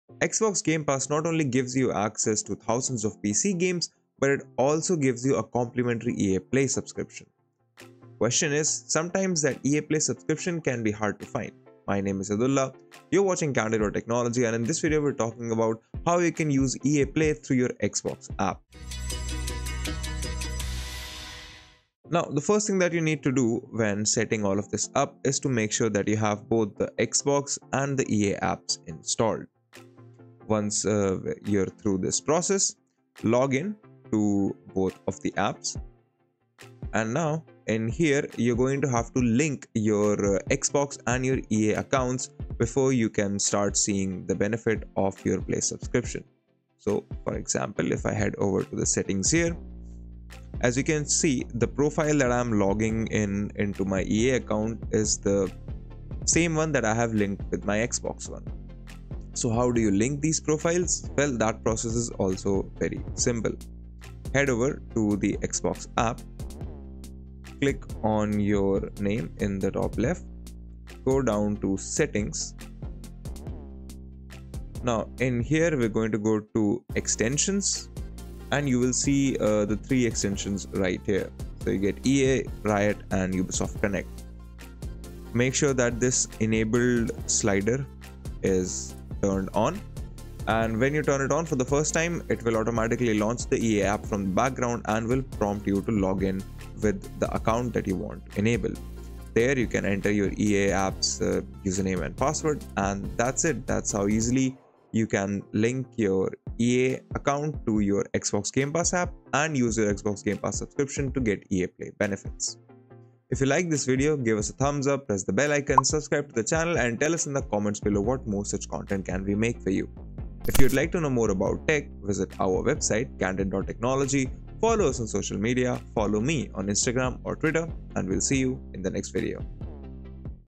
How to use EA Play through Game Pass on PC?
Here is how to use EA Play through Game Pass on PC. for more guides and tech explainers, reviews, lists, comparisons and more tech-related stuff.